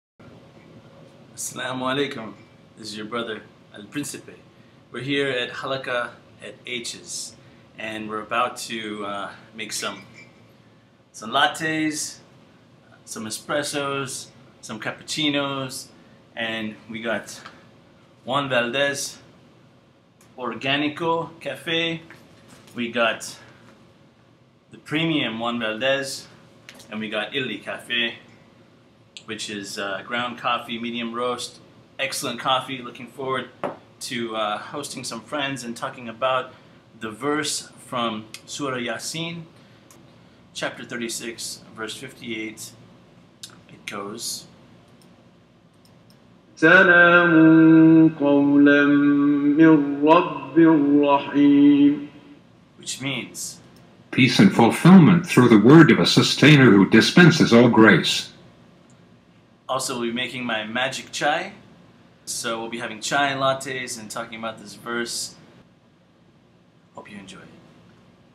Start With ONE Spiritual Verse: REFLECT, and SEE Differently
As-salaamu’alaykum translates to “Peace Be Upon You”

At my penthouse on the 80th floor of the John Hancock Building in Chicago. Known as “H’s” for “Hassan’s”.

Taking a moment to remember God Almighty and reflect on peace and humanity.

Specifically, the verse:

Salamun Qaulam Mir Rabbir Rahim, from Surah Ayat 58, Qur'an

(36:58) “Peace” shall be the word conveyed to them from their Merciful Lord.